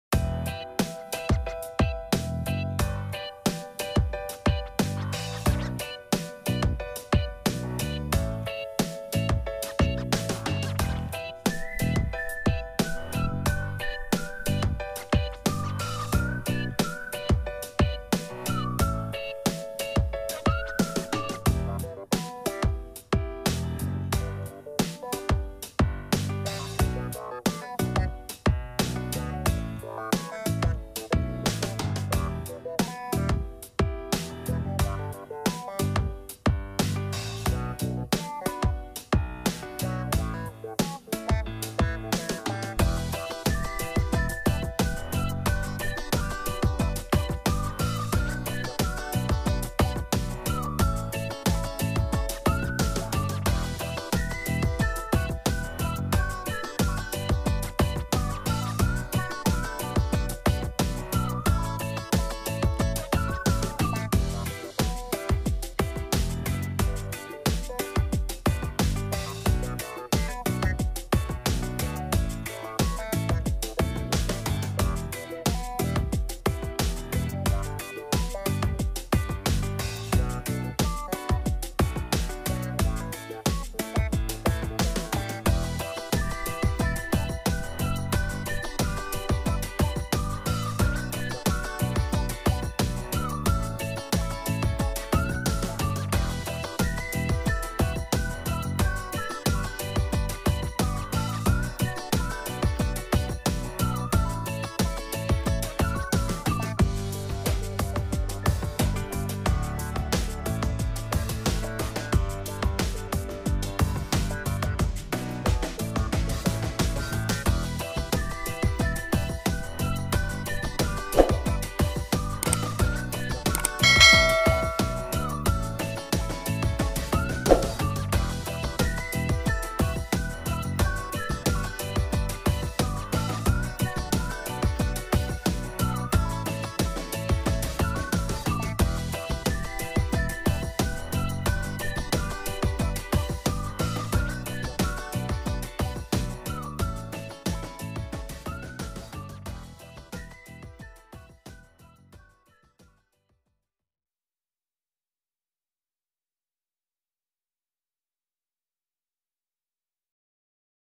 Cách gấp máy bay giấy bay mãi không rơi, máy bay giấy bay xa và thẳng | Gấp máy bay giấy #185
Video này nói về cách gấp Máy bay Giấy Cơ bản - Nếu bạn muốn Máy bay Giấy bay được 100 mét (hoặc 10.000 mét), hãy tập ném thẳng vào một ngày trời quang đãng.